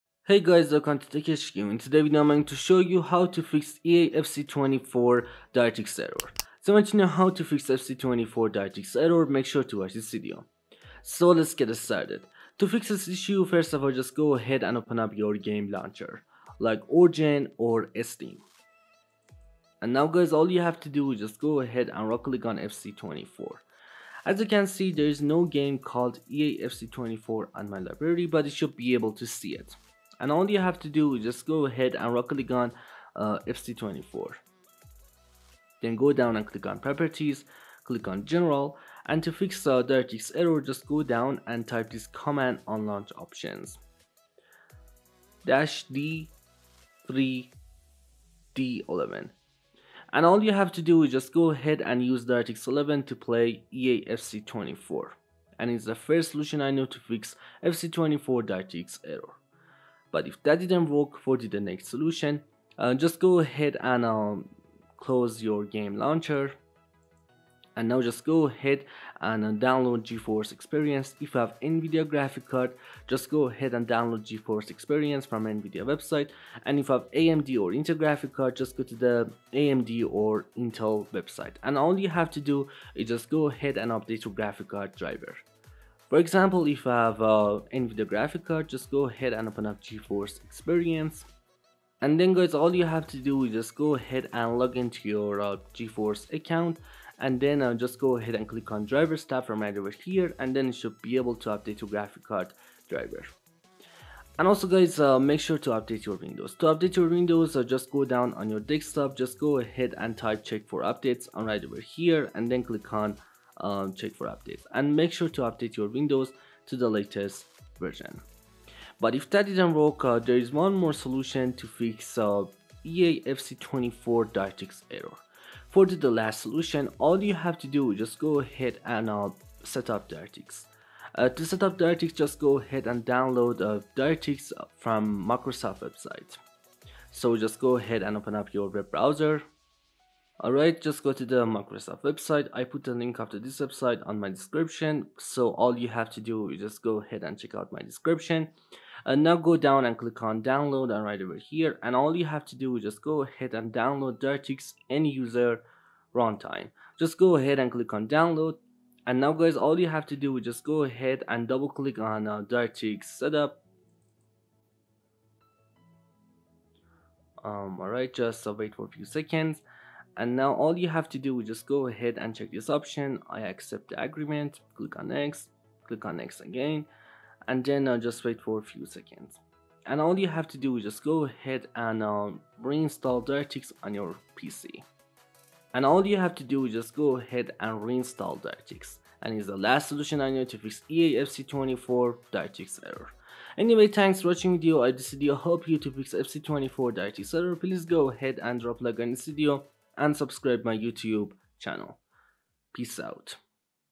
How To Fix FC 24 Directx Error | EAFC 24 Directx Error Fix
Hey everybody in this video I'm showing you How To Fix FC 24 Directx Error | EAFC 24 Directx Error Fix. so if you want to know how to fix fc 24 directx just watch this video.